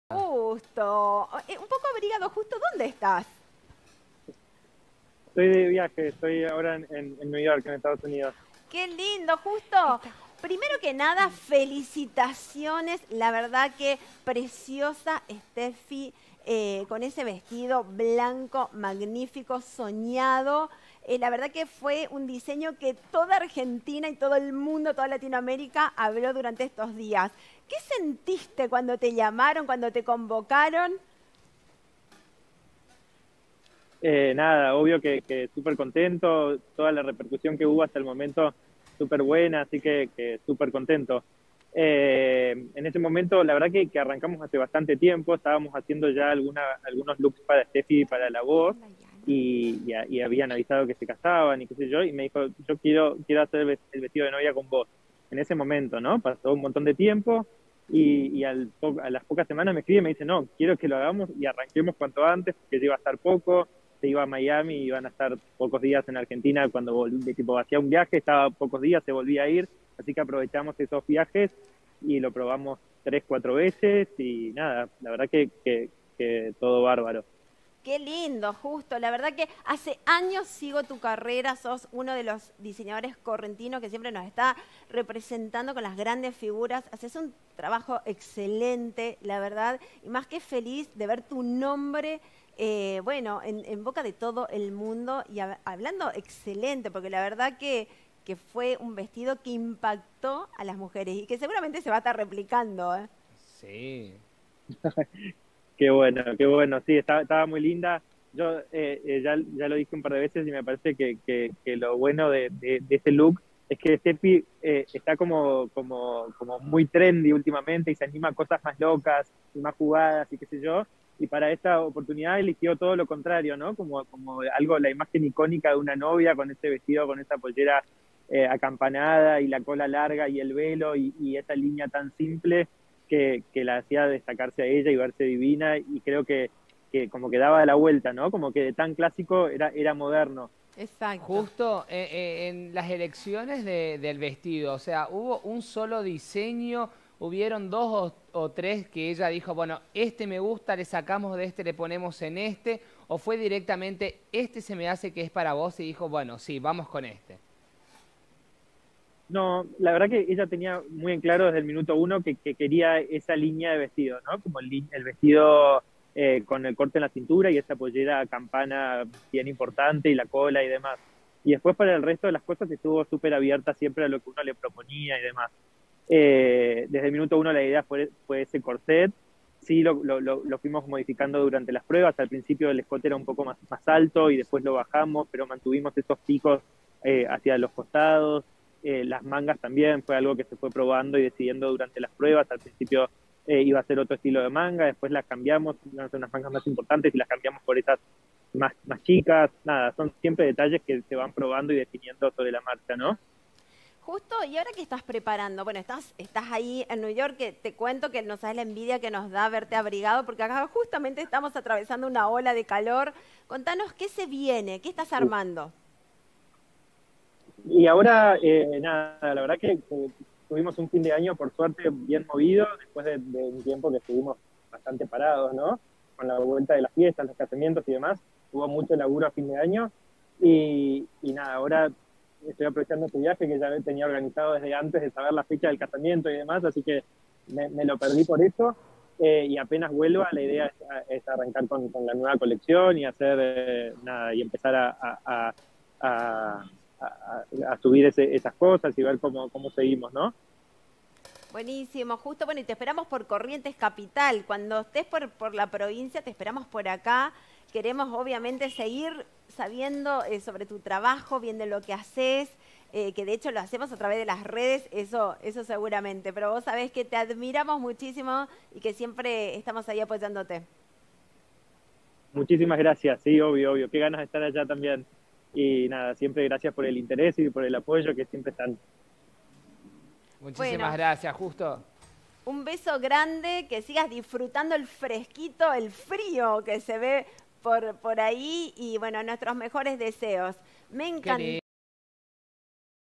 Diseñador correntino hace vestido de novia de boda internacional
Justo Ocampo es oriundo de CuruzúCuatiá y fue el elegido por Stefi Roitman para confeccionar el vestido que usó en su boda con Ricky Montaner 👰‍♀️🤩